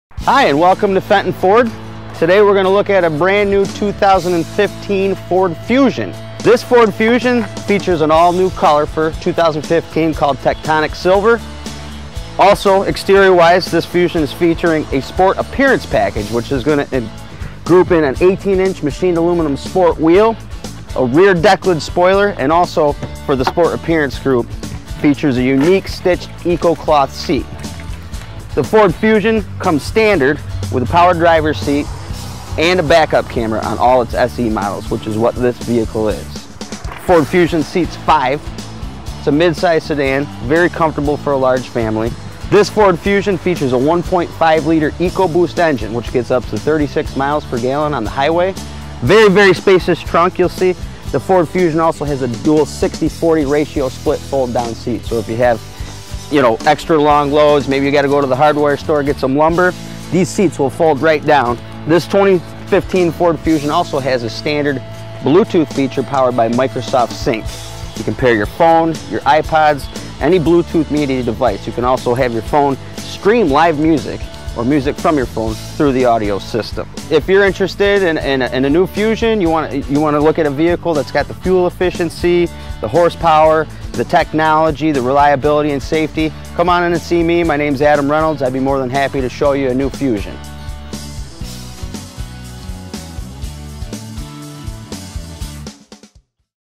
Adam Introduces the 2015 Fusion
Adam Reynolds does a walk around to show you the new 2015 Ford Fusion at Fenton Ford in Camden, NY!